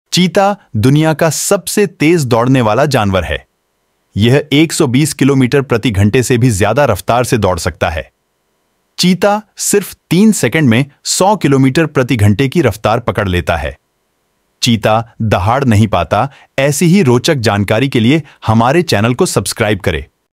दुनिया का सबसे तेज़ दौड़ने वाला जानवर है
चीता दुनिया का सबसे तेज़ दौड़ने वाला जानवर है. यह 120 किलोमीटर प्रति घंटे से भी ज़्यादा रफ़्तार से दौड़ सकता है.
चीता सिर्फ़ तीन सेकंड में 100 किलोमीटर प्रति घंटे की रफ़्तार पकड़ लेता है.
चीता दहाड़ नहीं पाता,चीता,घास के मैदानों की रक्षा करता है और उसे नुकसान पहुंचाने वाले जानवरों को खदेड़ देता है.
चीता, बिग कैट फैमिली का सबसे सुडौल और तेज़ रफ़्तार दौड़ने वाला जानवर है.